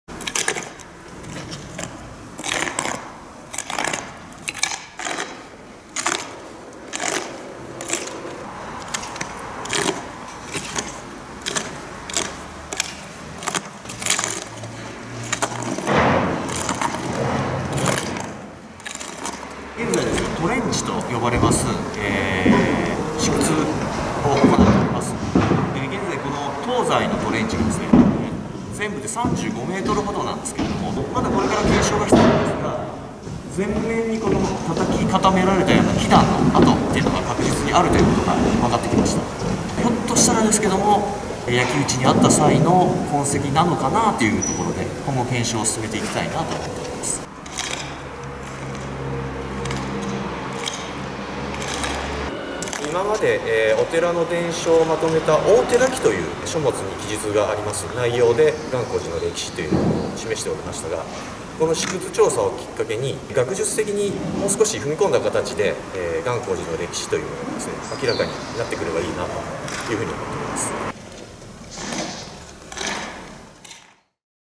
信玄が焼き打ち？ 「証し」探して発掘 岐阜・願興寺
解体修理中の#国重要文化財 「願興寺本堂 」（岐阜県御嵩町 ）の敷地で、町教育委員会が#発掘調査 を進めている。寺伝書には兵火で二度も焼失し、地域の人たちが再建したなどと書かれており、町教委の担当者は「寺の開創や兵火の時期、再建規模などがわかるかもしれない」と話している。